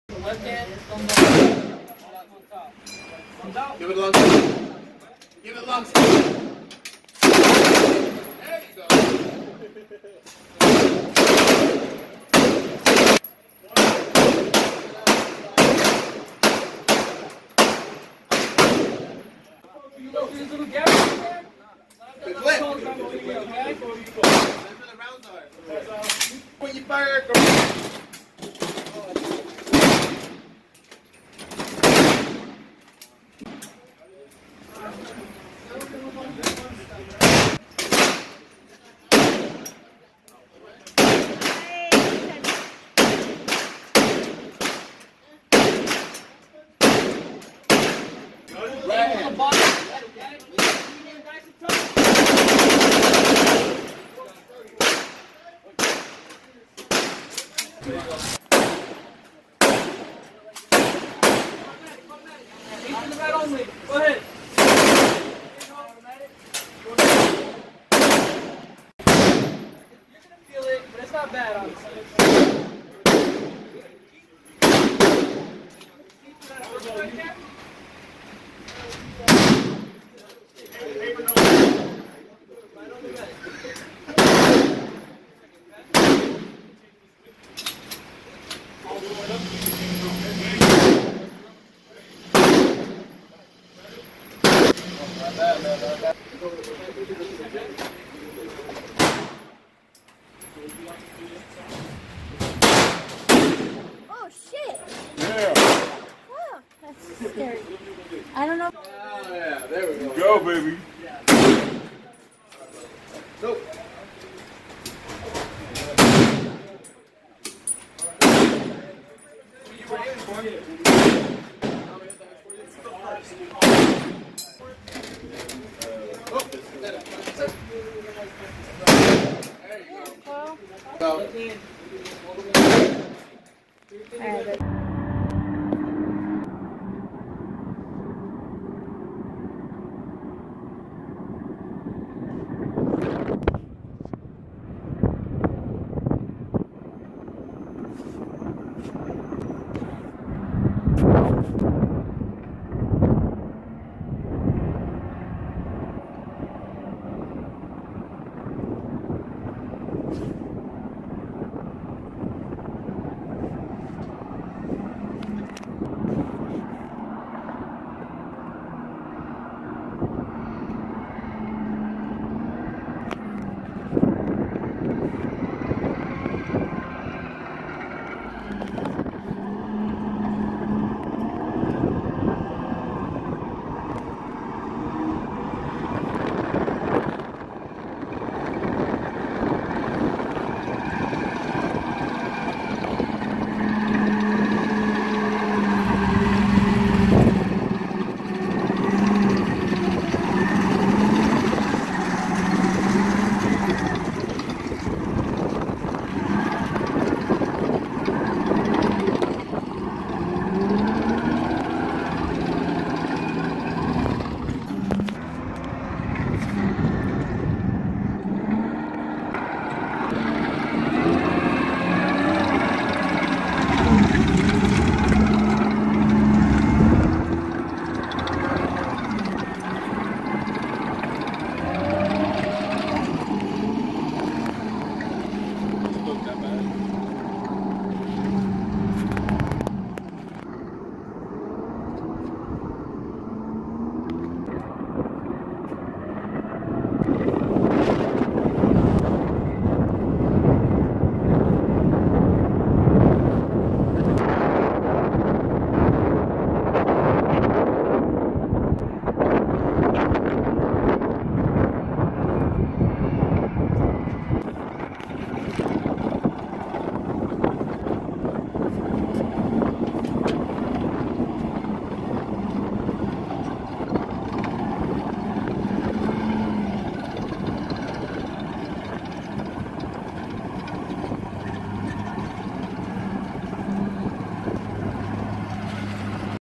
Gun Range and military transporter Las Vegas Nevada. Address in description.
At the Batterfield Vegas here in Las Vegas Nevada. Address for the place, 2771 S Sammy Davis Jr Dr, Las Vegas, NV 89109

Me and my Family went on my Husband's Birthday of May 2021. My Husband was so happy he got to drive the tank I call it.
He got to shoot at the gun Range.
They also pick you up from your hotel or whatever location you give them. They also take you back as well.

The tank was around $500 total for having extra passengers to ride along.
The gun shooting I believe was like $250 to $500 they have package deals. I started to shoot but got scared and couldn't do it. My Husband took over and finished my rounds so it wasn't a waste. He had the best 50th birthday ever.

We had a really good week in Vegas. I would love to go back and experience more fun things.
They also have a helicopter ride to the Grand Canyon.
That's where I would love to do. I believe that one cost $200 a person. Eventually hopefully in a year that's my goal is to take the helicopter ride to the grand canyon and bring the whole Family again.

Also comment and let me know what other type of adventure videos would you like to see.